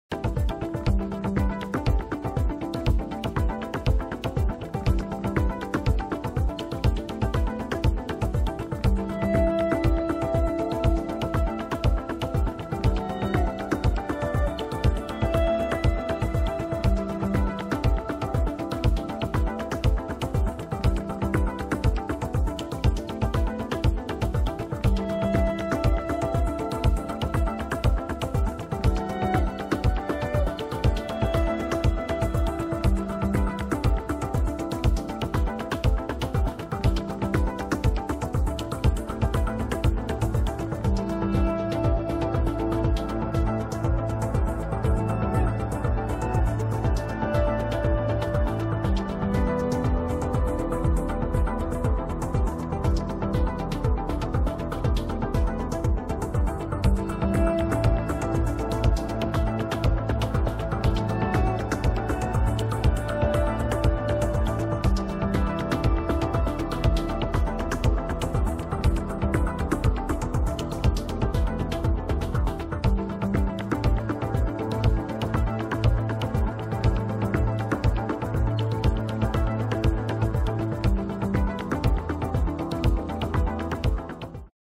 Nicoletti Romeo Sectional Sofa at ModernManhattan.com
Romeo comes in Dark Grey Italian leather to take the comfort and style of your home to a higher level. Equipped with quiet and easy-to-use reclining mechanism.